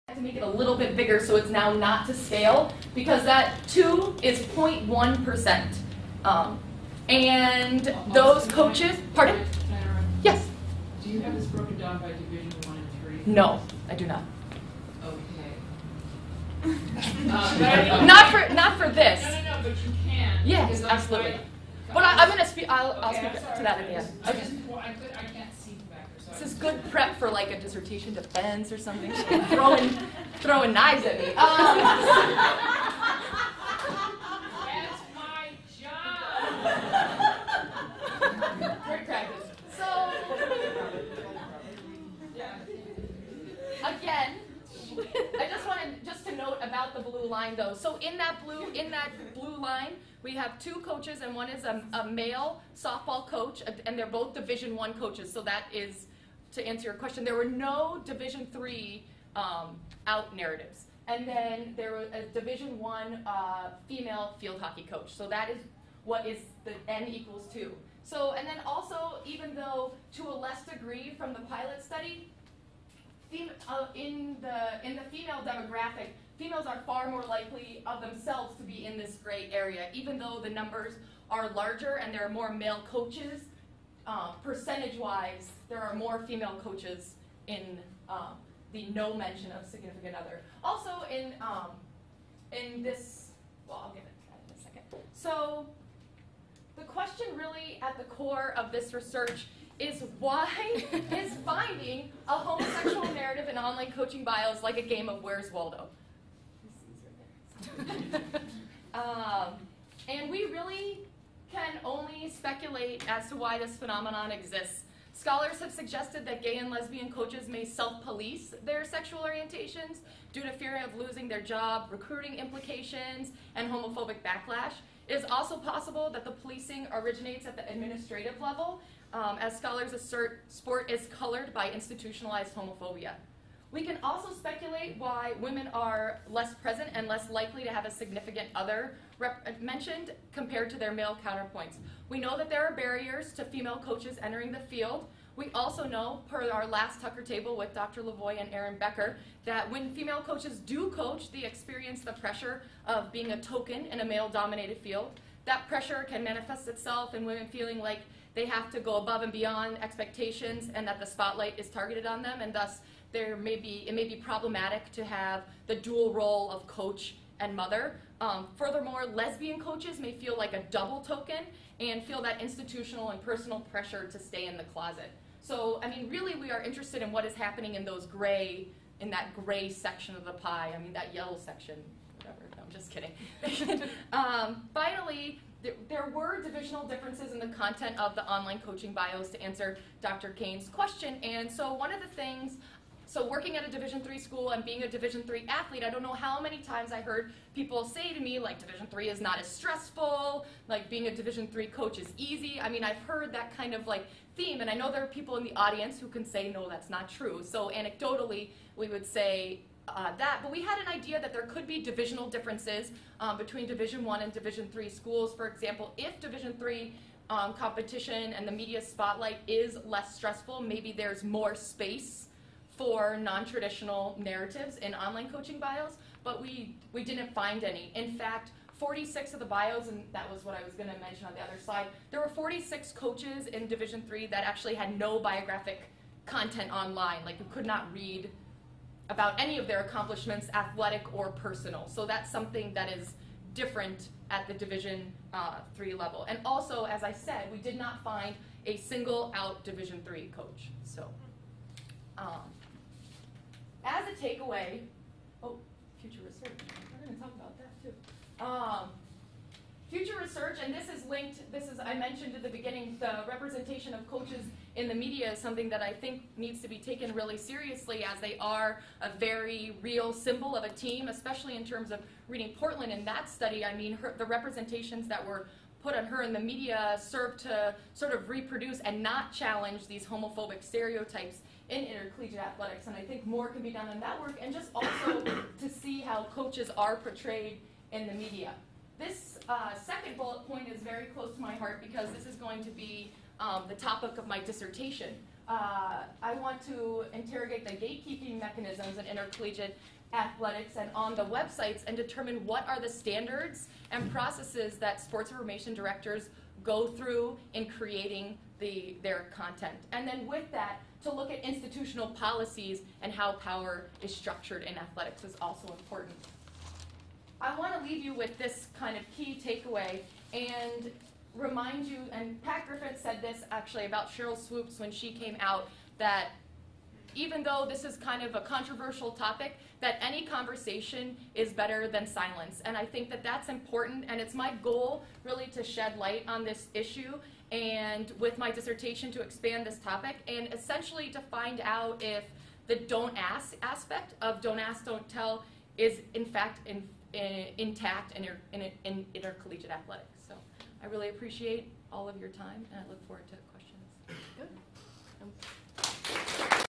Tucker Table, 2010 April, Austin Stair Calhoun
Research Presented by Austin Stair Calhoun, Kinesiology doctoral student-- University of Minnesota, at The Tucker Table, April 30, 2010. "Framing with Family: Examining Online Coaching Biographies for Heteronormative and Heterosexist Narratives." Discussion
[69]